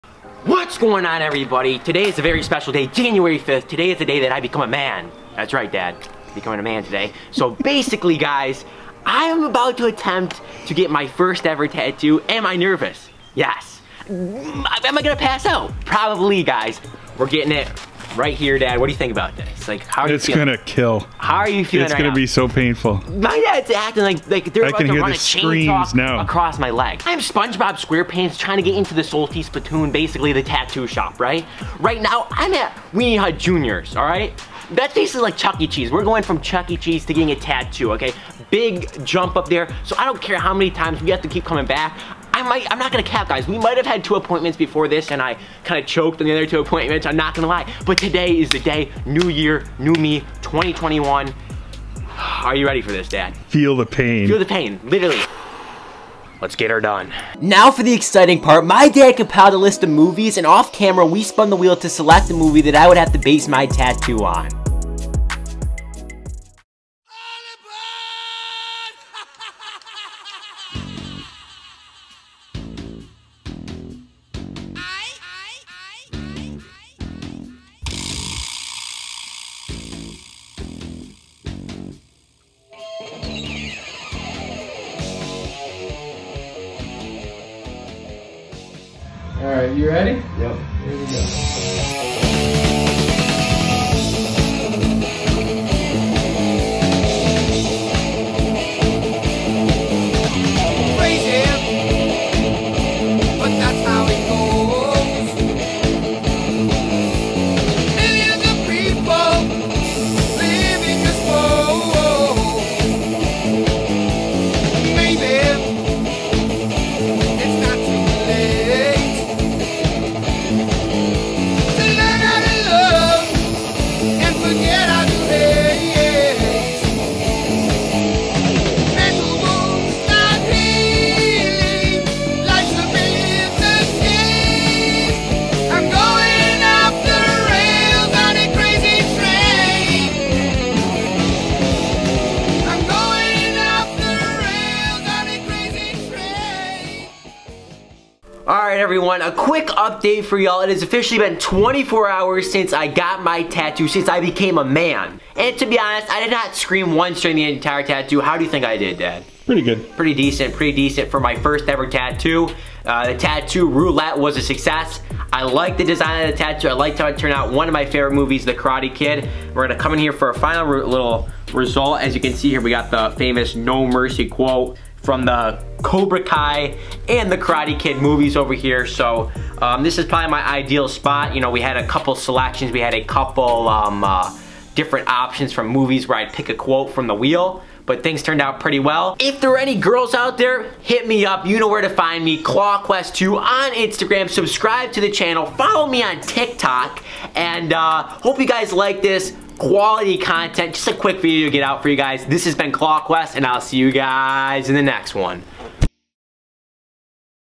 Mystery Tattoo Roulette Challenge.. *Painful*
In this video we spin a wheel and I get my first Tattoo!!